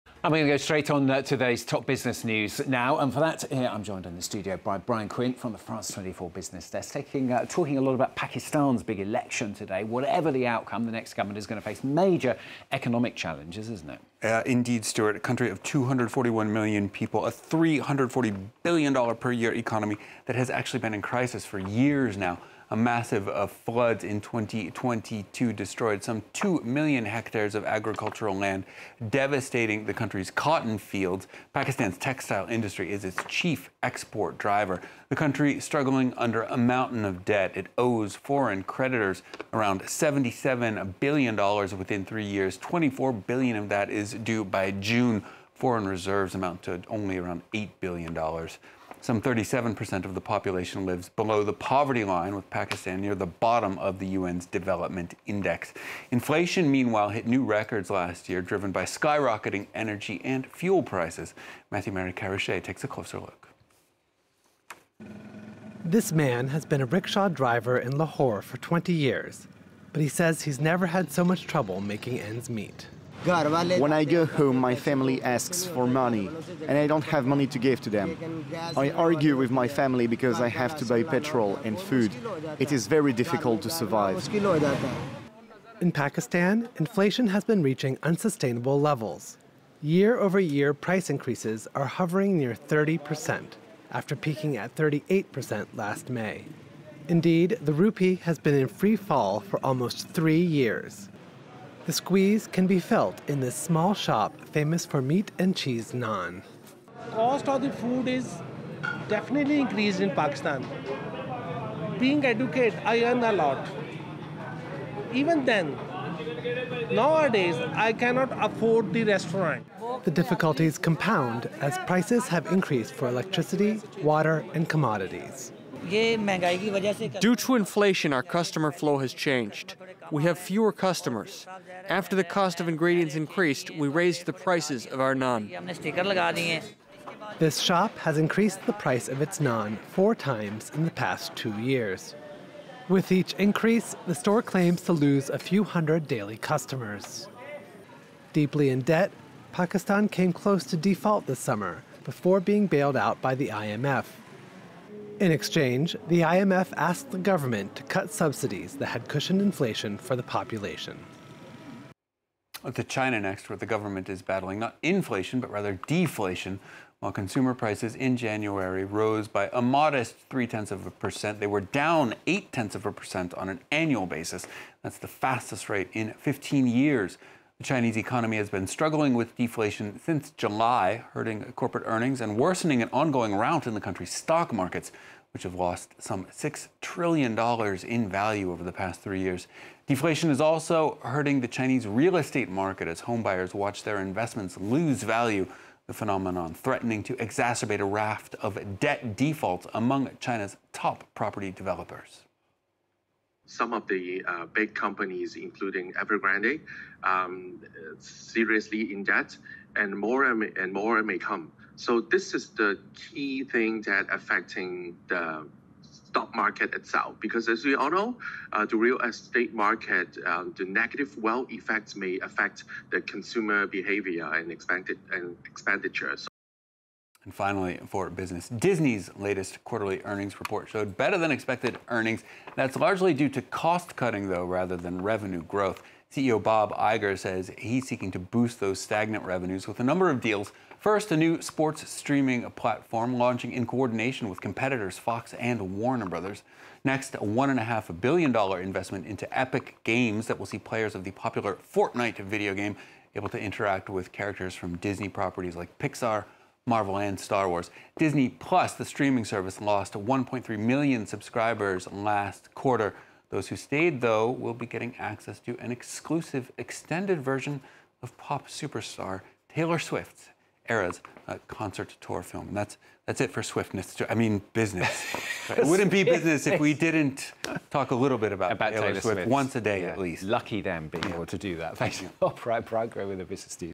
Pakistanis head to polls amid mounting economic instability • FRANCE 24 English
With high inflation, massive foreign debt and widespread poverty, Pakistan's economy has been in crisis for years, presenting major challenges for its next government. We take a closer look. Also, China battles deflation and a stock market meltdown. Plus, Disney looks to boost revenues with a sports streaming platform, video games and Taylor Swift.